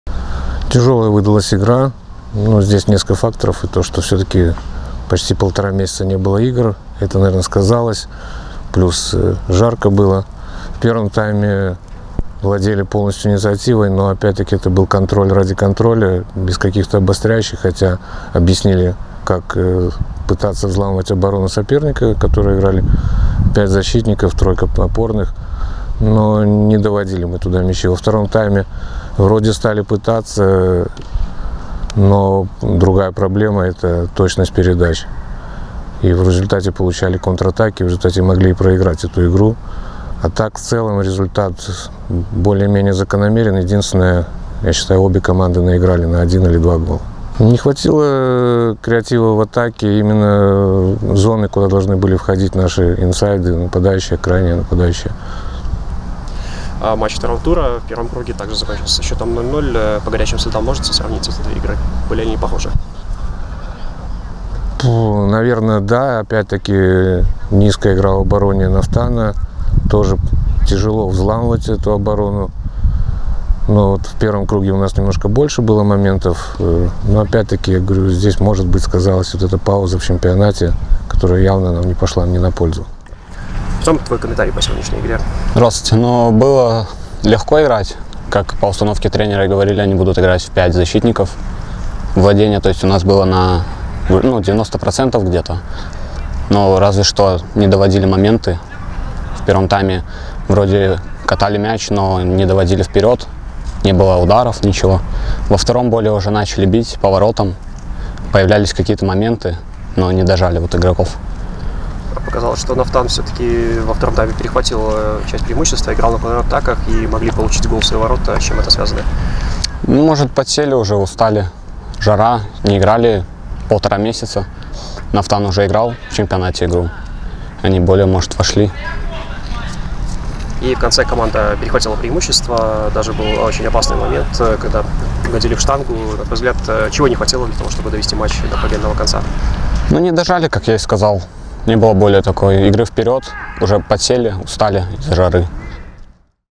Андрей ЮСИПЕЦ и Артём ПОЛУШКО | Комментарии после матча с резервистами ФК «Нафтан»
Послематчевые комментарии главного тренера дублирующего состава «Гомеля» Андрея Юсипца и защитника «зелено-белых» Артёма Полушко.